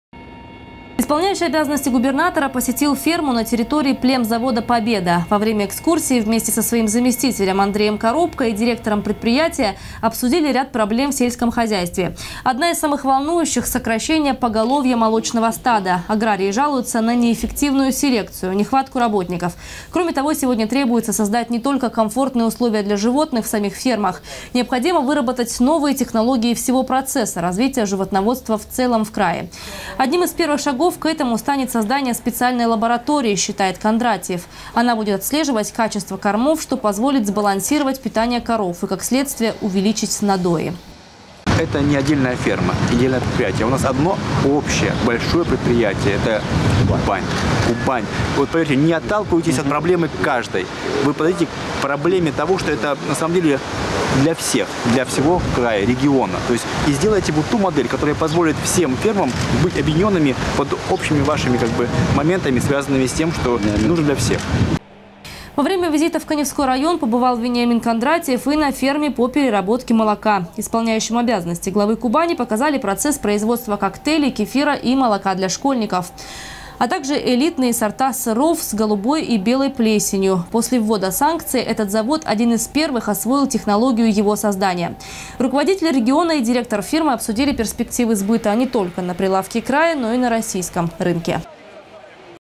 На Кубани создадут лабораторию анализа кормов для крупного рогатого скота. Новости Сочи Эфкате
Такое решение сегодня принял Вениамин Кондратьев. Рабочий день руководителя региона прошёл в Каневском районе.
Сочи. Новости.
Телеканал Эфкате.